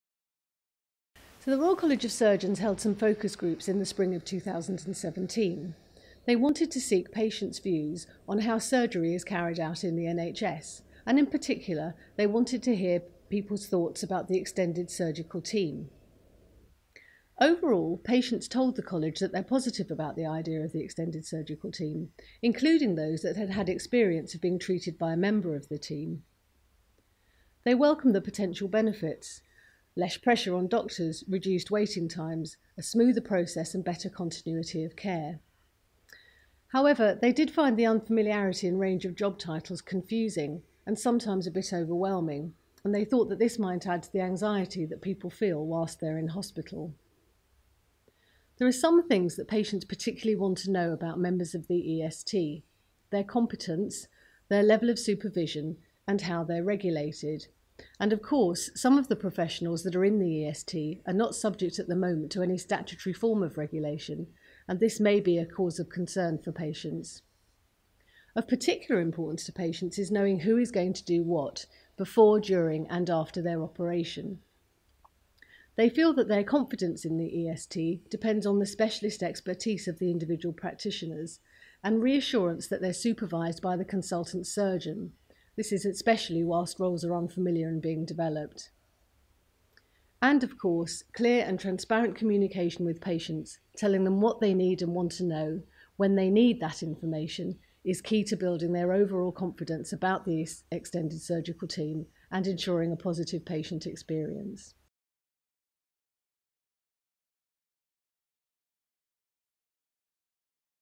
The surgical care team: a patient perspective
The Royal College of Surgeons is committed to supporting members of the extended surgical team (EST). Well managed use of the wider team, with its mix of staff and skills, ultimately benefits patients, surgeons in training and the whole surgical team.

This video with Caroline Browne illustrates how patients feel about the surgical care team and describes what further information patients would like to receive regarding their surgical team.